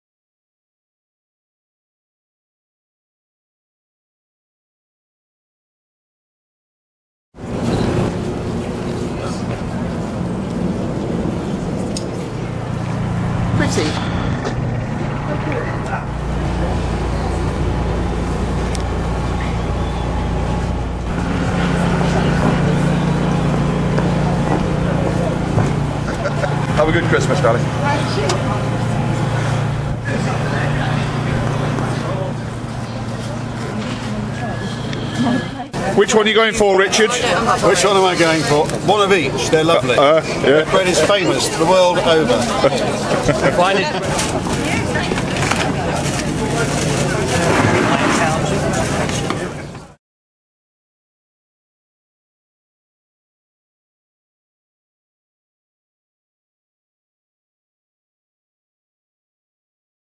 Camberley Christmas Eve Farmers Market 2011
Camberley bursting with Chritsmas Spirit in Park Street